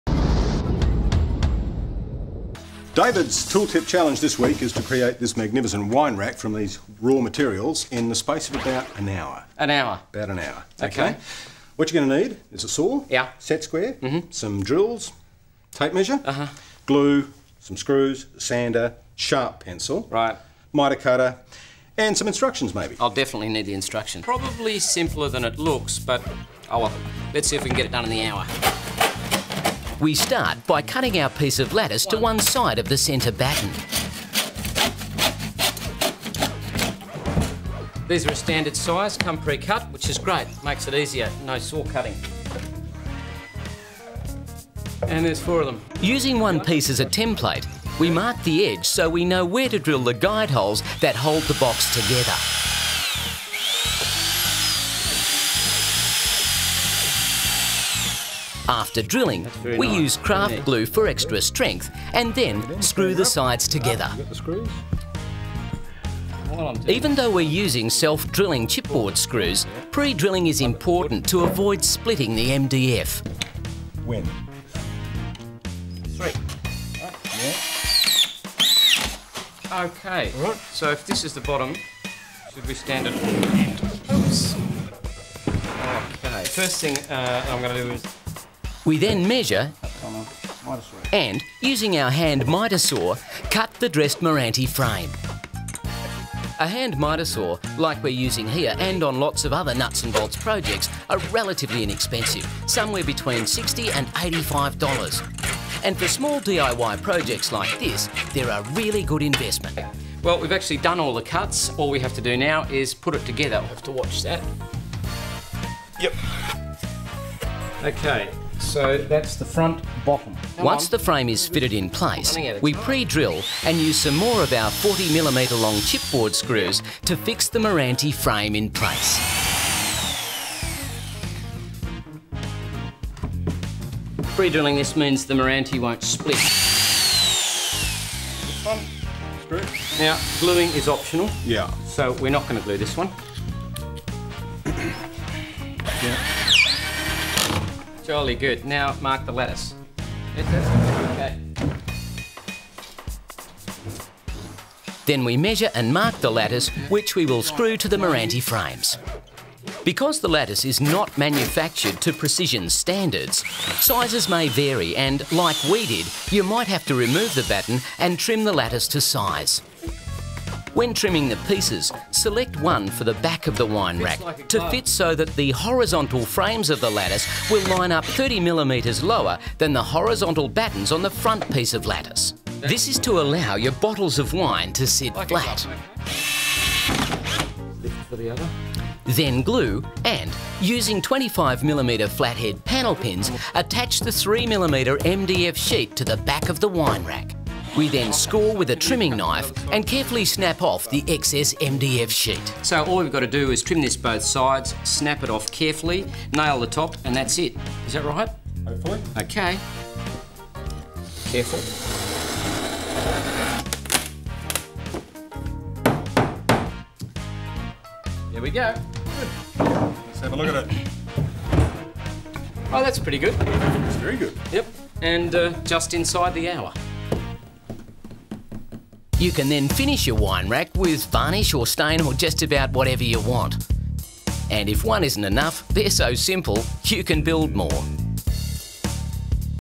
How to Build a Wine Rack
We are going to make this magnificent wine rack from some lattice using some simple hand tools in about an hour.